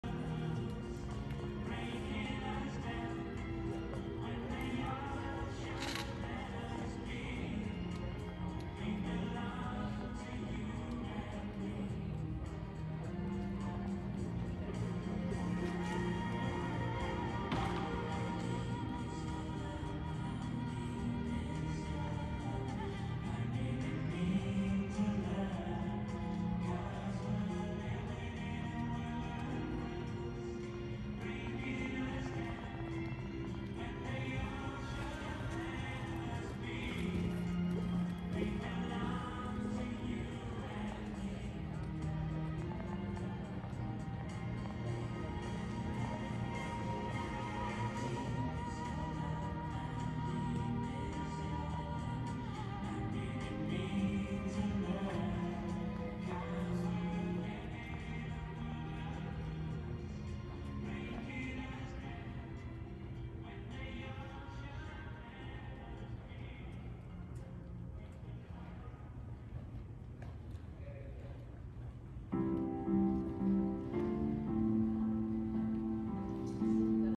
85. WIHS hunter phase, 1st place
WIHS equitation hunter phase
1st place
Lexington country horse show 2
February 2022
“Lucky Man,” 7 year old jumper